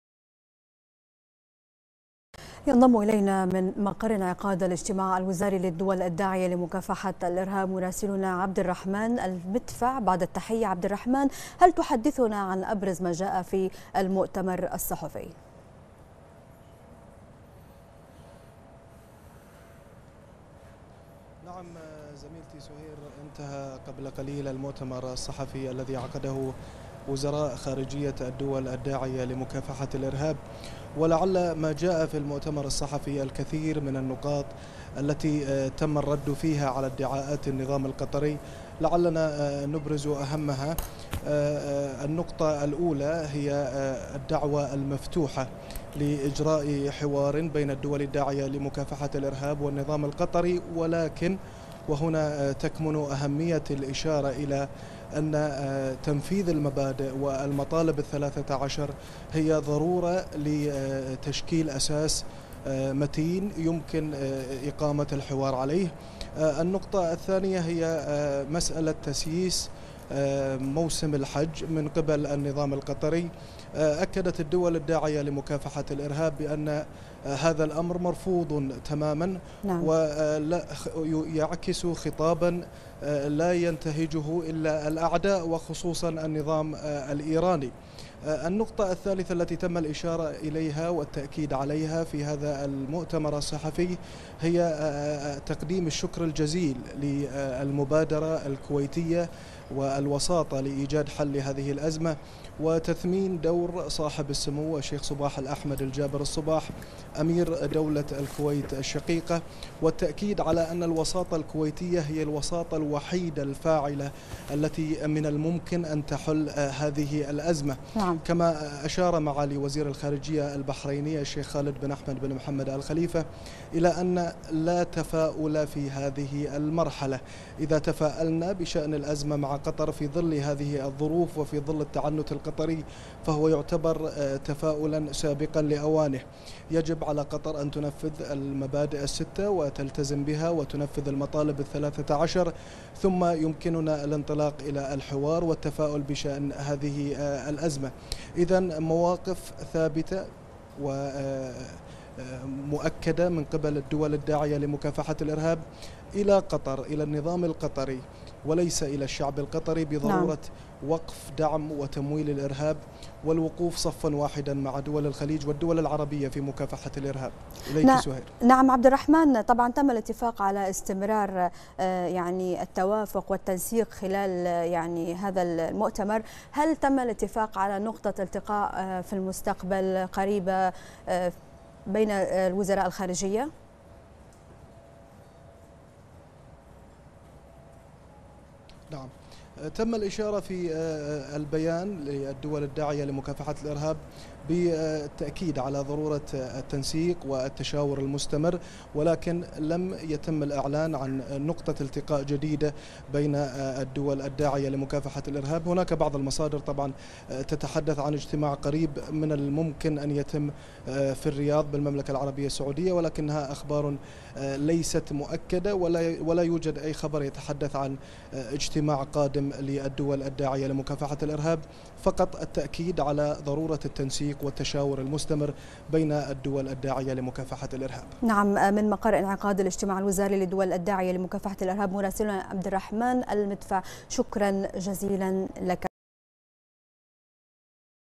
البحرين: مداخلة / عبدالرحمن المدفع مراسل مركز الأخبارحول إجتماع الدول الداعية لمكافحة الإرهاب
البحرين: مداخلة / عبدالرحمن المدفع مراسل مركز الأخبار حول إجتماع الدول الداعية لمكافحة الإرهاب